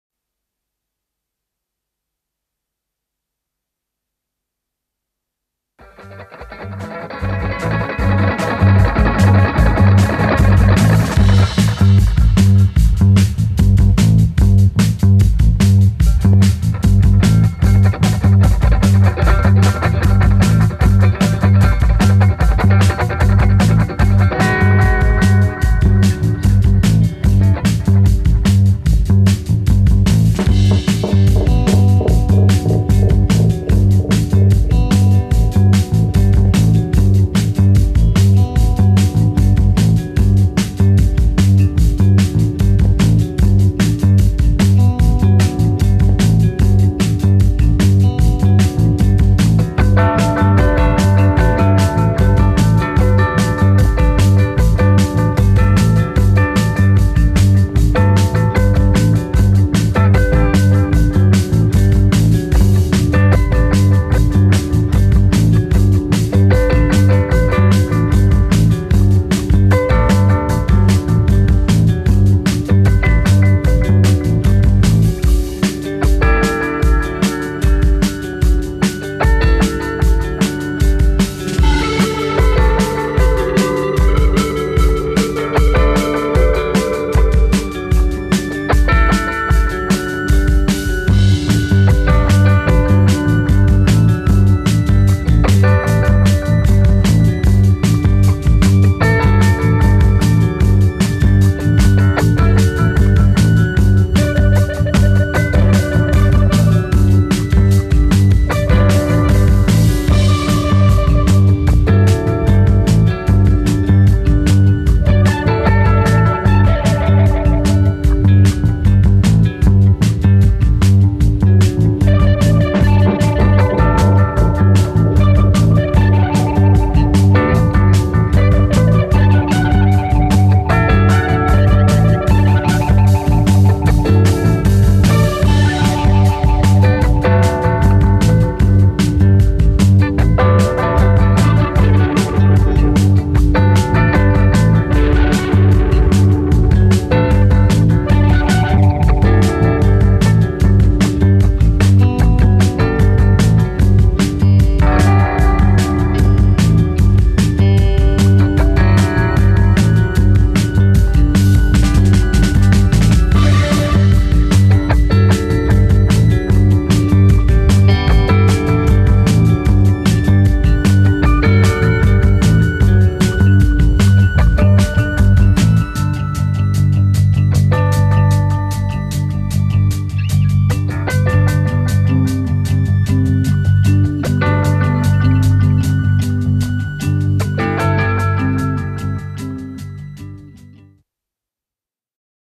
Panorami d'autore a Siena
Le belle locations di film come "Io ballo da sola", "Il paziente inglese" e "Il gladiatore" nelle splendide campagne senesi.